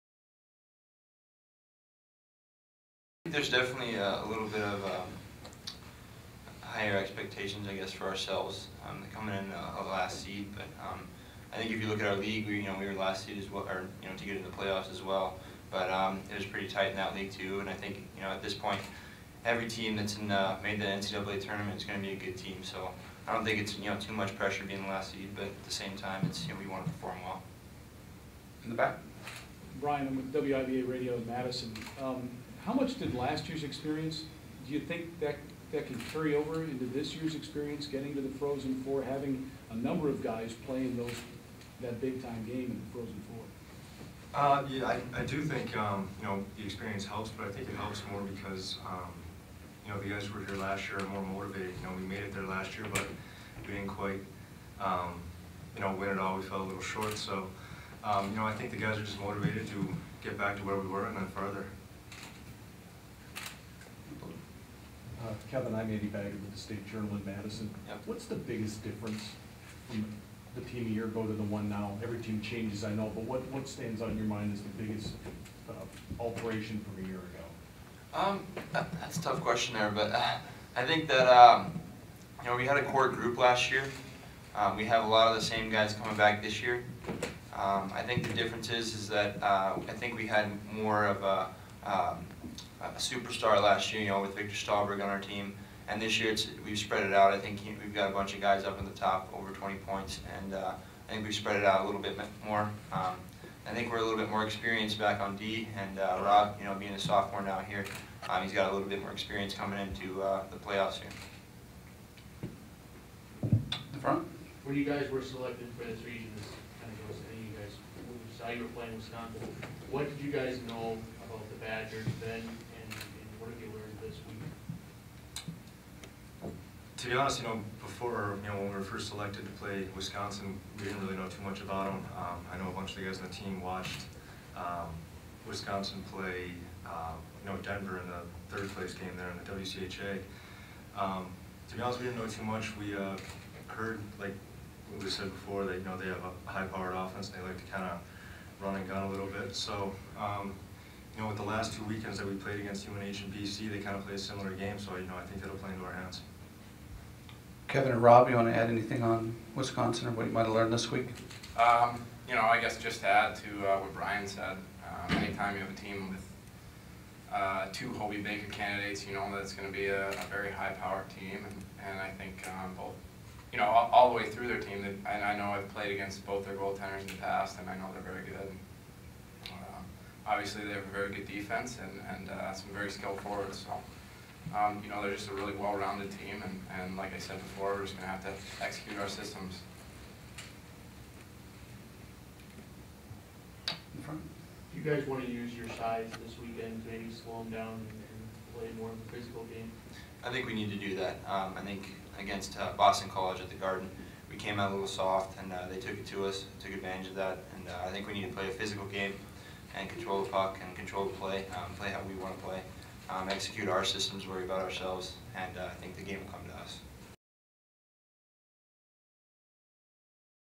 Men's Hockey NCAA West Regional: Media Day - Student Athletes
Senior Brian Roloff, Junior Kevan Miller, and Sophomore Rob Madore spoke with the media at the Xcel Energy Center in St. Paul, Minnesota. The Catamounts practiced for one hour Thursday before their press conference.

The Catamounts take on the #5 Wisconsin Badgers Friday night at 9 ET on ESPNU in the West Regional Semifinals.